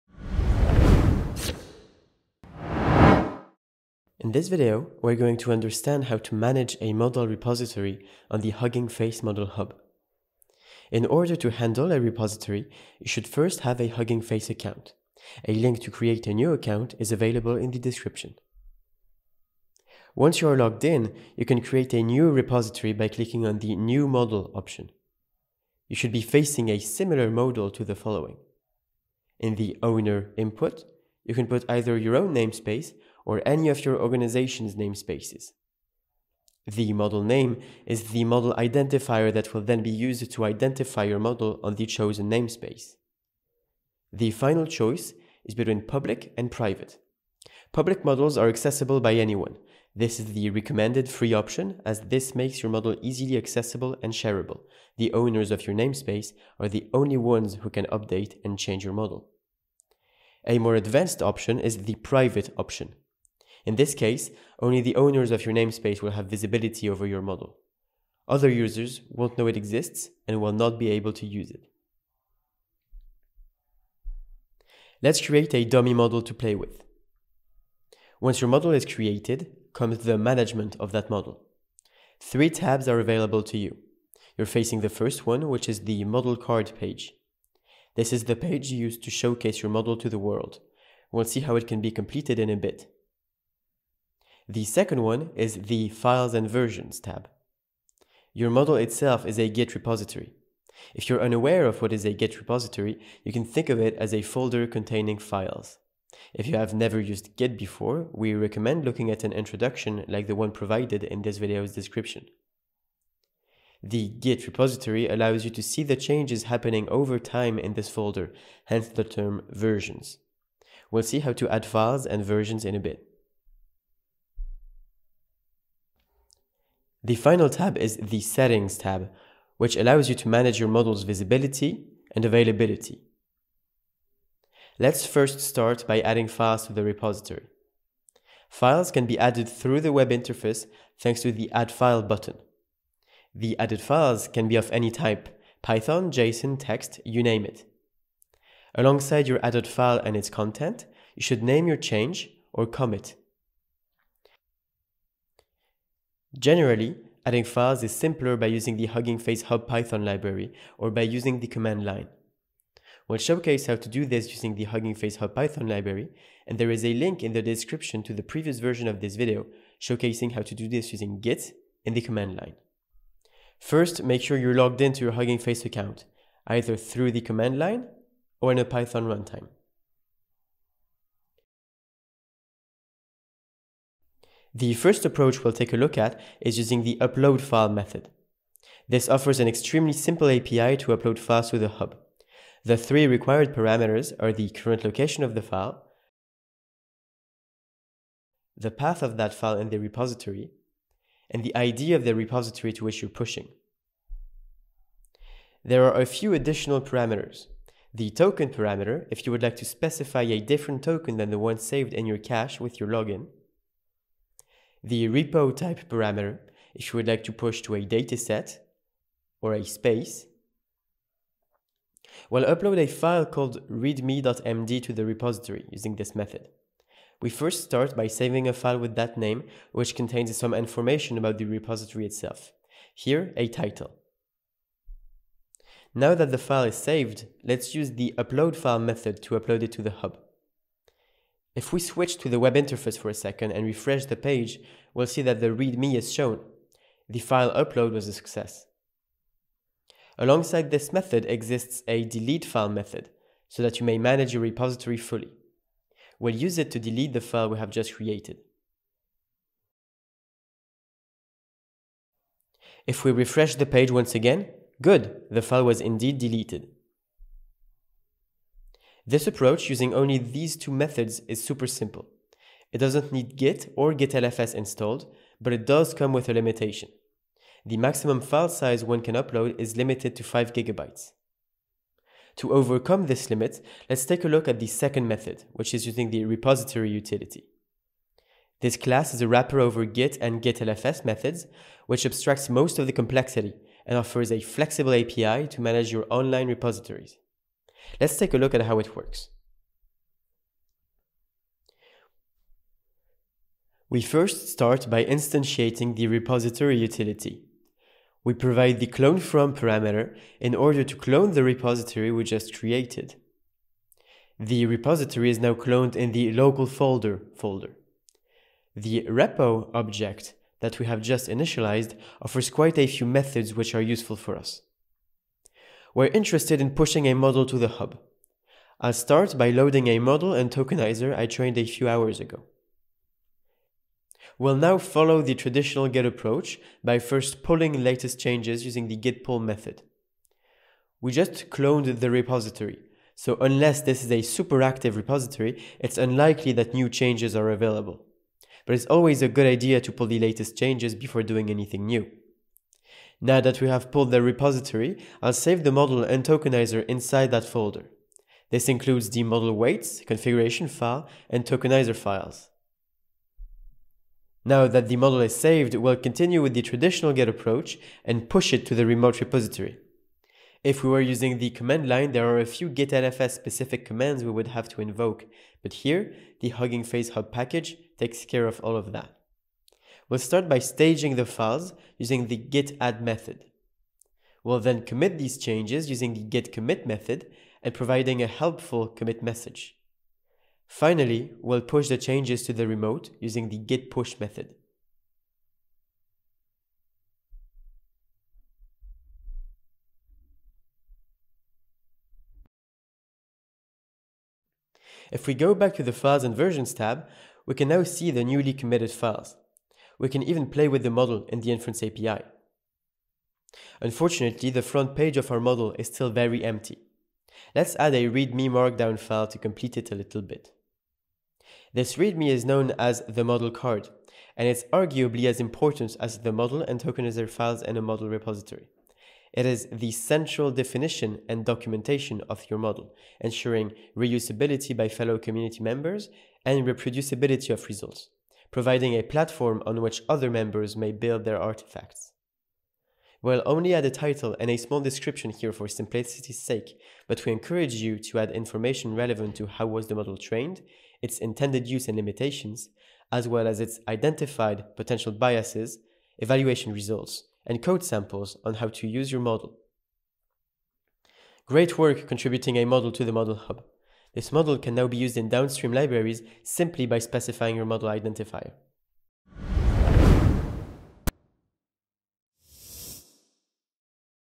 Managing a repo on the Model Hub
Learn to manage a repository on the Hugging Face Model Hub. This video covers the creation of a repo, file additions through the web interface as well as through the command line.

You will need a Hugging Face account to manage a repo on the Model Hub.

In order to follow this video, we recommend learning about git and git-lfs beforehand.

Have a question? Checkout the forums